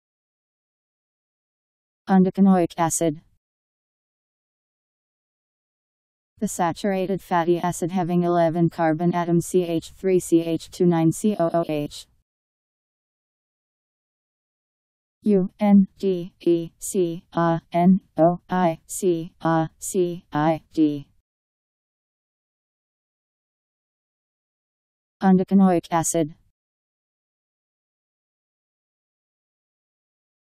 Undecanoic acid Meaning
Video shows what undecanoic acid means. The saturated fatty acid having 11 carbon atoms - CH3(CH2)9COOH.  Undecanoic acid Meaning. How to pronounce, definition audio dictionary. How to say undecanoic acid. Powered by MaryTTS, Wiktionary